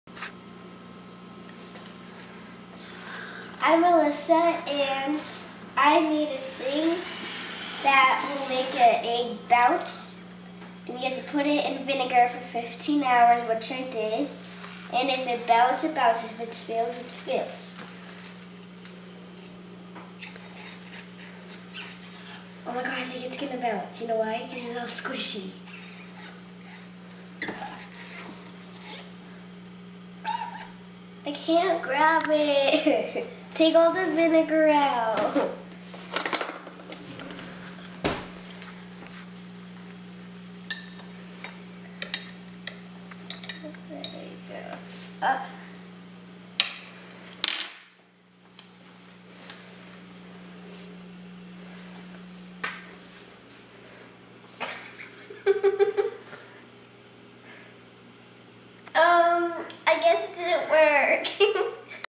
Bouncy Egg Experiment - A Really Fun and Easy Science Experiment to do at Home!
Just add a raw egg to a cup of vinegar.  Leave it overnight sitting in the vinegar and the hard shell begins to turn squishy.  The egg is bouncy.  But don't try to bounce it from too high in the air or watch what will happen.  You will have a big mess to clean up.  This is a really fun and easy experiment to do at home!

Here is the science behind this experiment!  Egg contain calcium carbonate.  This is what makes the shell of the egg hard.
Vinegar is an acid known as acetic acid. When calcium carbonate (the egg) and acetic acid (the vinegar) combine, a chemical reaction takes place!  Carbon dioxide (a gas) is released, this is what the bubbles are in your vinegar/egg mixture.  When the egg is removed from the vinegar, the shell is soft and squishy.  This is because all of the carbon was removed from the shell in those little bubbles!

After the experiment, if you leave your egg sitting out it will begin to turn hard again.  This is because the softened egg will "steal" back the carbon dioxide that it lost while sitting in the vinegar.